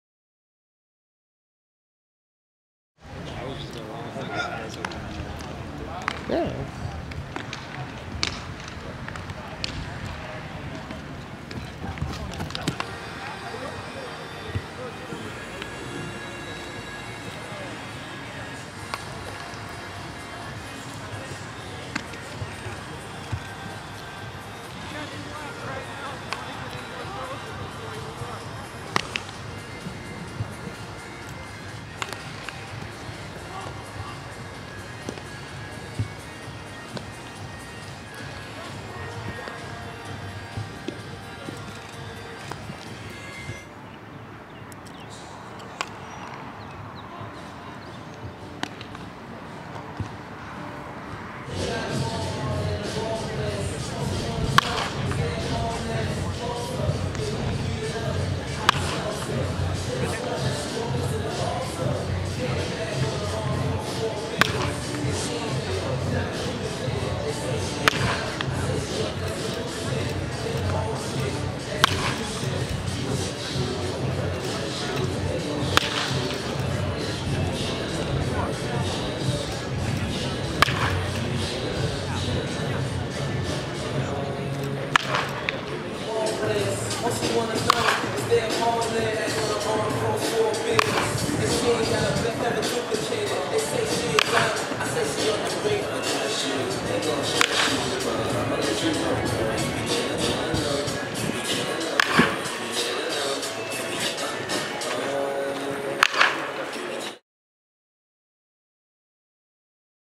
Heliot Ramos Prospect Video, OF, Las Lomas Potros Class of 2017
Heliot runs 60, fields and bp at PG National Underclass